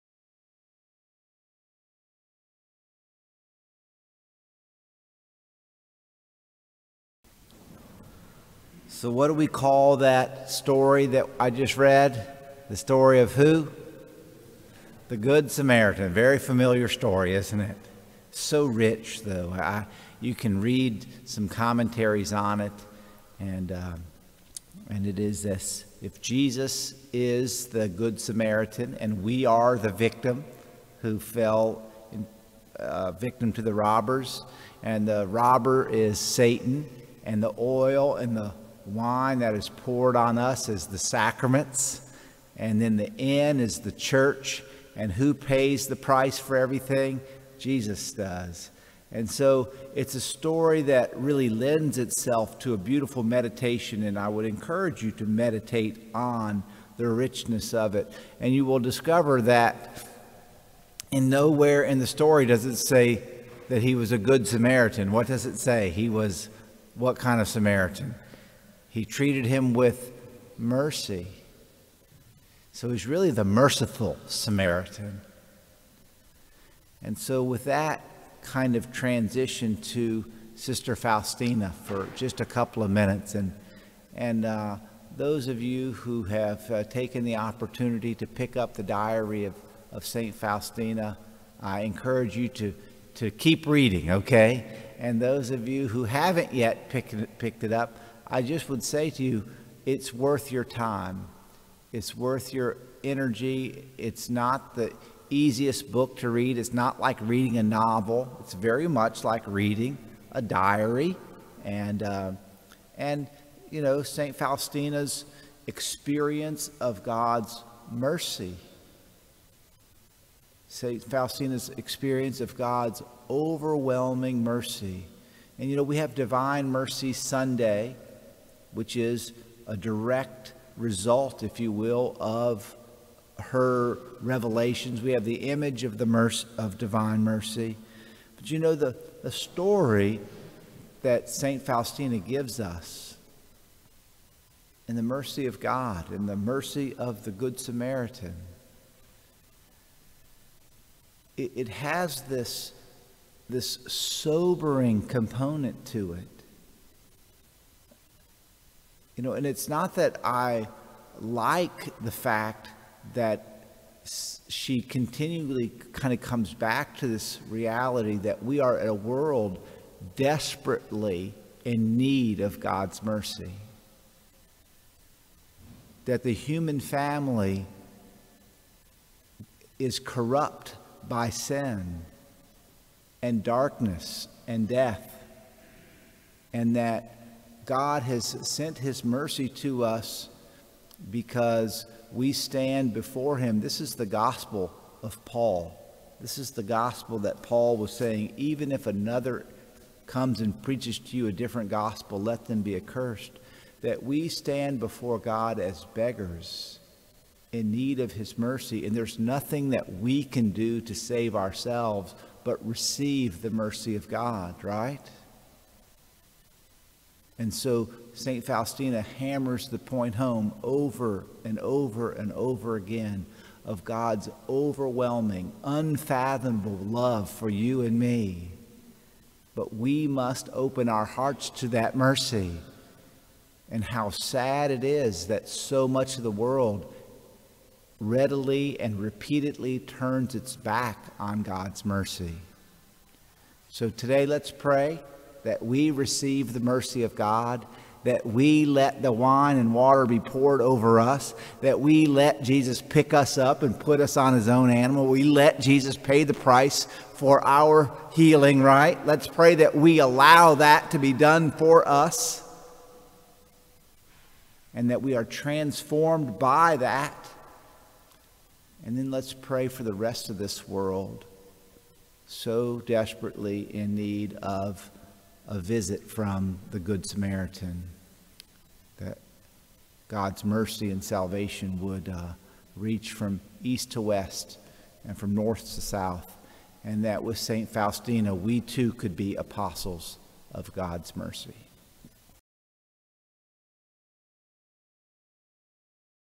"The Merciful Samaritan"
Father Michael O'Connor's homily from Mass on Monday, October 5, 2020 at Our Lady of the Gulf Catholic Church in Bay St. Louis, MS